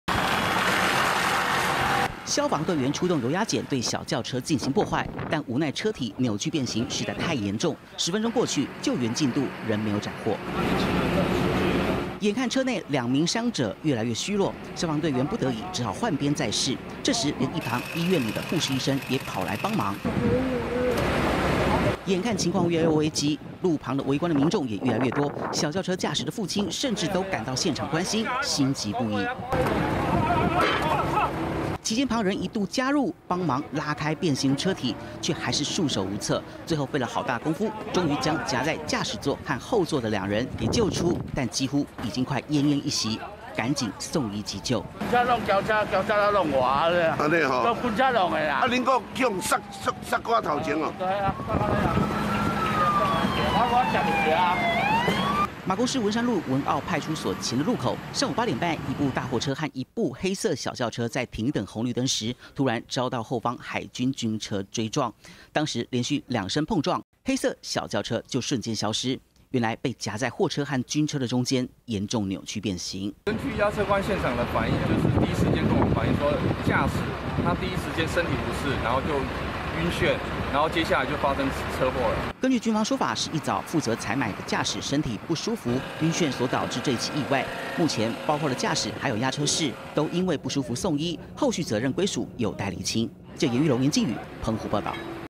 20171012中天新聞 駕駛暈眩！ 軍車追撞 轎車遭擠扁釀3傷
澎湖馬公發生海軍軍車追撞意外，現場一輛轎車被卡在貨車和軍車中間，兩人受困車內，由於車體嚴重扭曲變型，警消是花費好大一番功夫，才將人救出，初步了解，是因為軍車駕駛開車途中暈眩，因此發生這起追撞意外。